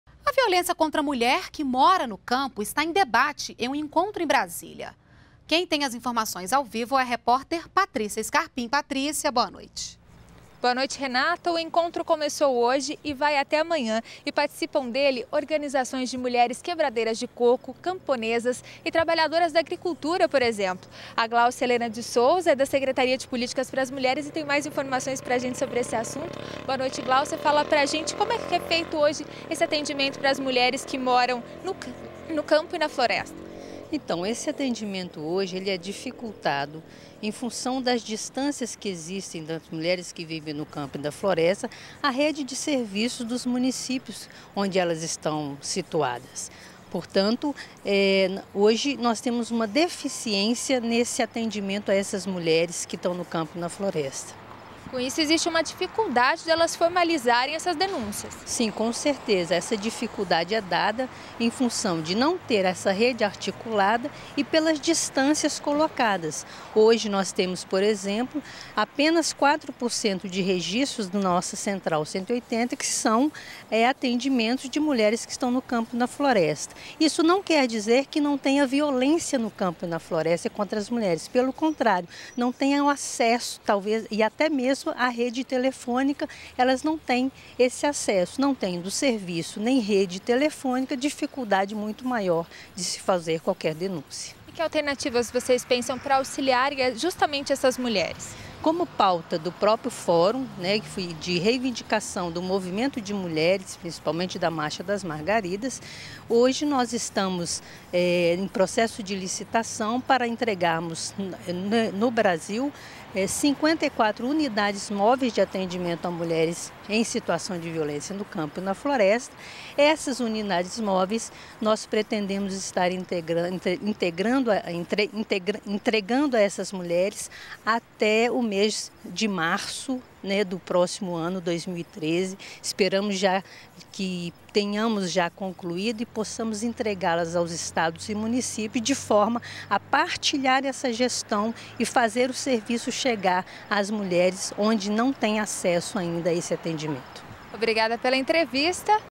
Fórum debate formas de enfrentar à violência contra a mulher do campo e da floresta
NBR NOTÍCIAS - 18.09.12:  A Secretaria de Políticas para as Mulheres da Presidência da República (SPM) promove, em Brasília, o Fórum Nacional de Enfrentamento à Violência contra a Mulher do Campo e da Floresta. A coordenadora da Rede de Atendimento da SPM, Gláucia de Souza, aponta que o atendimento a mulheres dessas regiões é dificultado devido às distâncias para acessarem os serviços nos municípios. Atualmente, apenas 4% das queixas feitas no Disque 180 são de mulheres do campo. Para ampliar o acesso dessas mulheres, 54 unidades móveis de atendimento à mulher vítima de violência serão entregues até março de 2013.